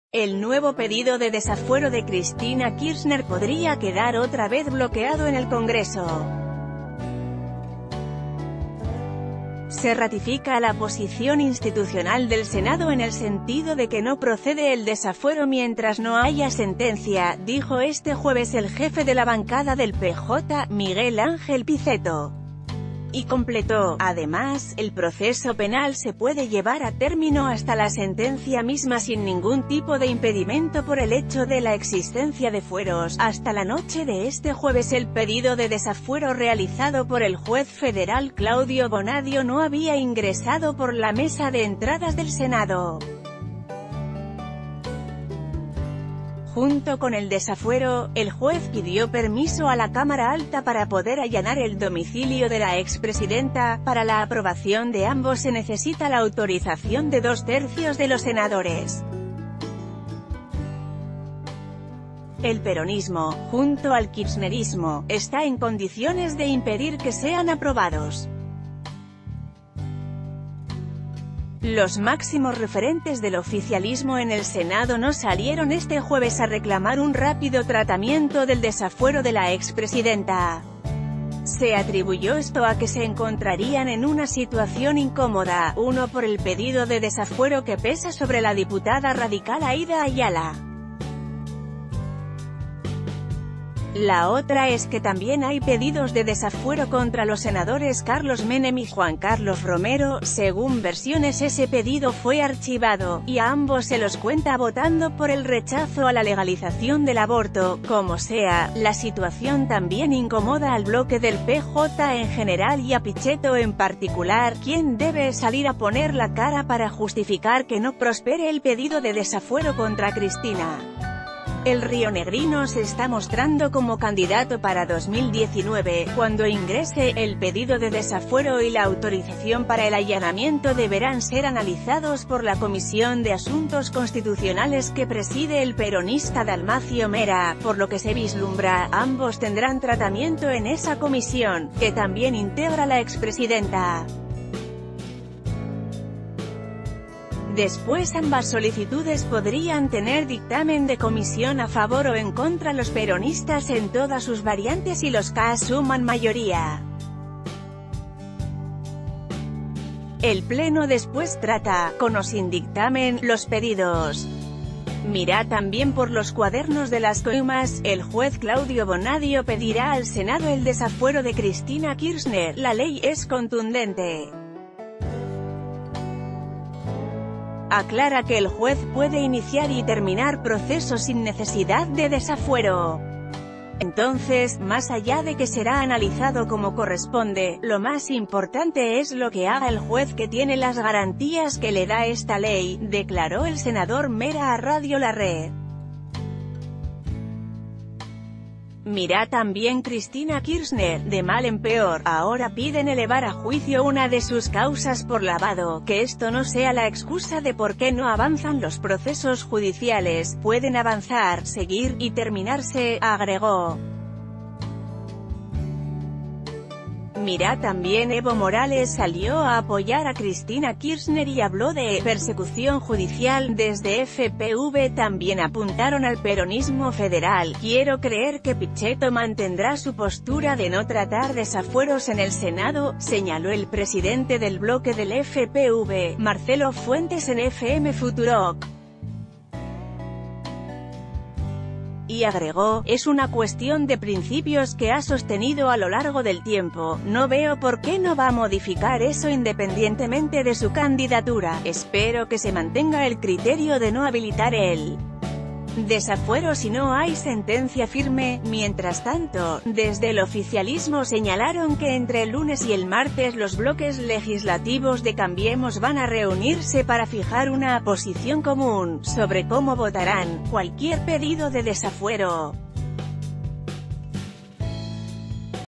El PJ mantiene su postura: no apoyarían el desafuero de Cristina Kirchner sin sentencia firme
Eric Goodwin